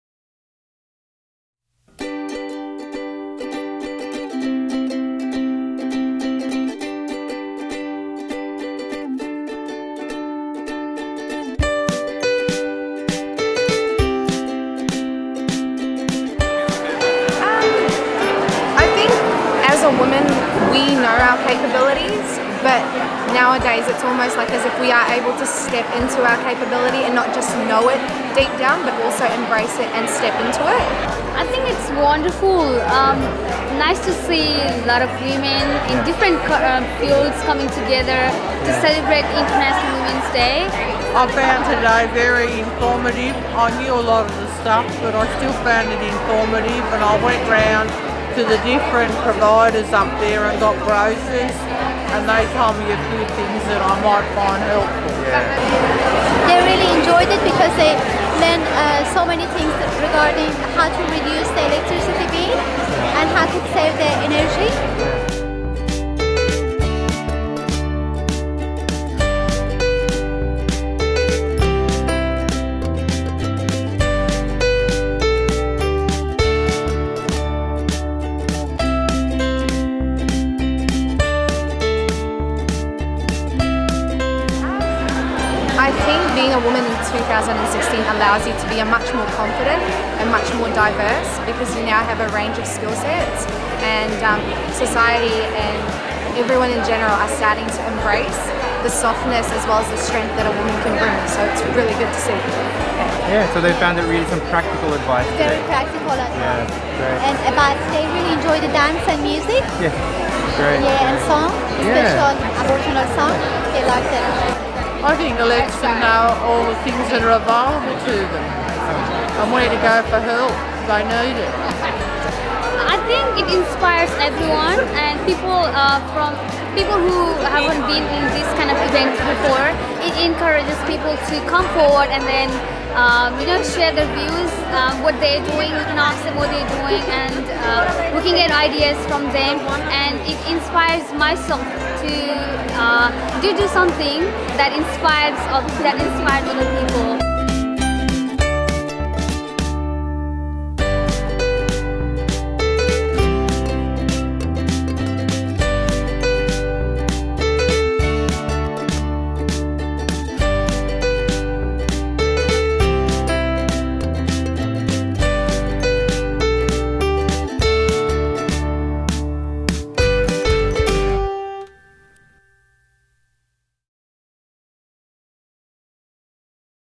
International Women's Day 2016
Penrith’s International Women's Day celebration was a tremendous success with more than 160 women from all walks of life coming together to share stories and friendship at a free event on Tuesday 8 March, 2016.